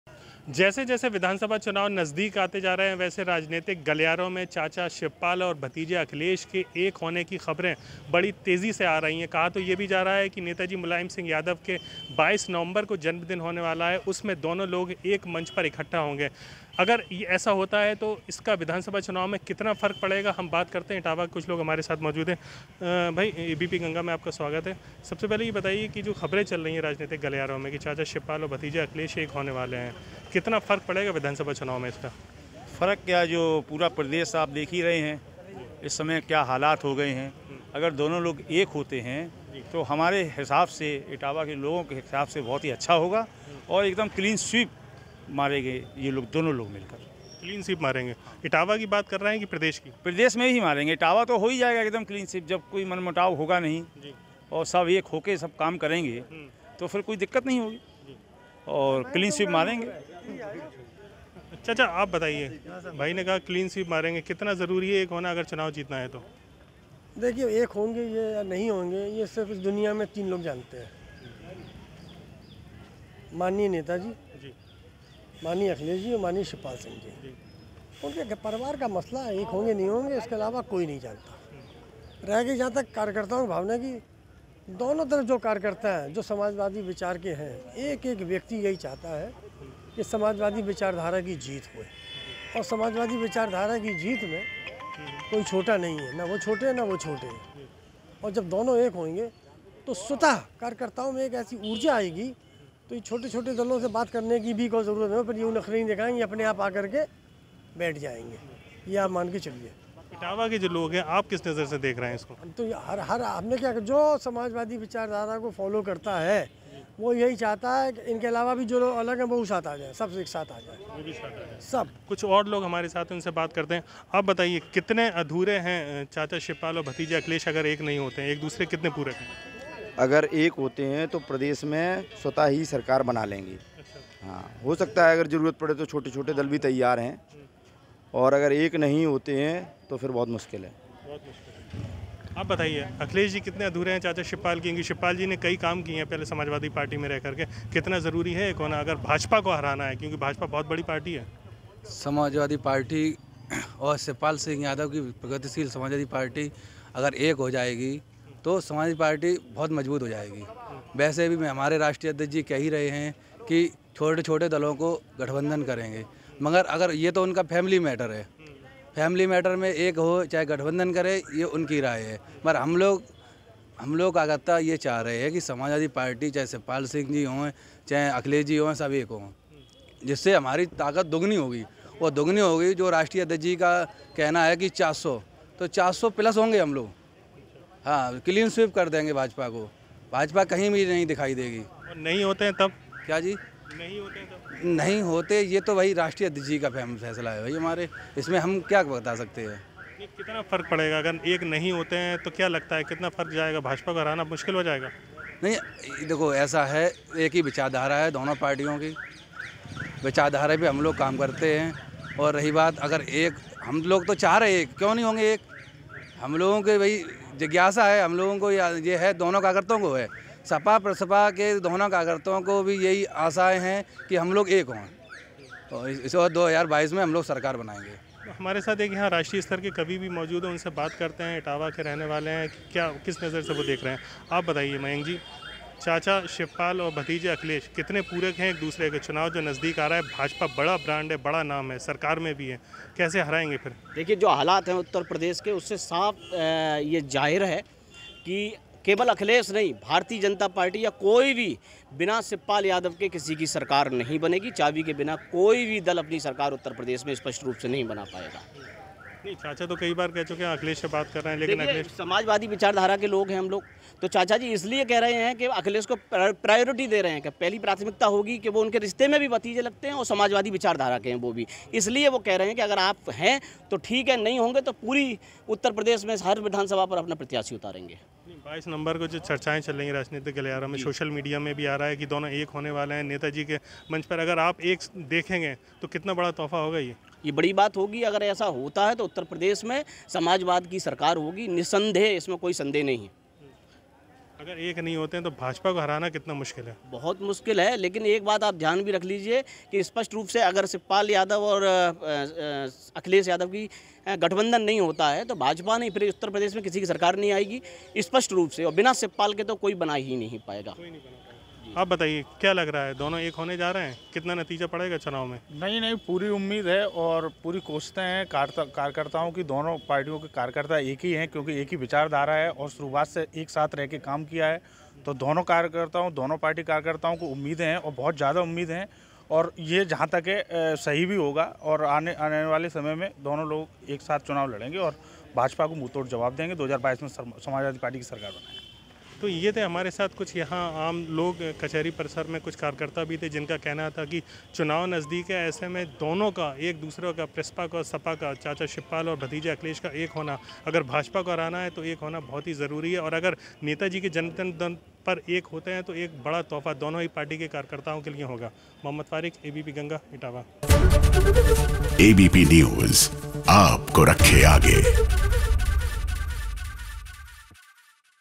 Shivpal Yadav-Akhilesh Yadav की मुलाकात पर इटावा के लोगों ने ये क्या बोल दिया!
इटावा राजनीतिक गलियारों में चाचा शिवपाल और भतीजे अखिलेश का नेता जी मुलायम सिंह यादव के जन्मदिन पर एक होने की चर्चाओं के बीच इटावा के लोगो और दोनो ही पार्टी कार्यकर्ताओं को एक होने की उम्मीद कहाँ ऐसा होता है तो भाजपा को हराने में होगी आसानी..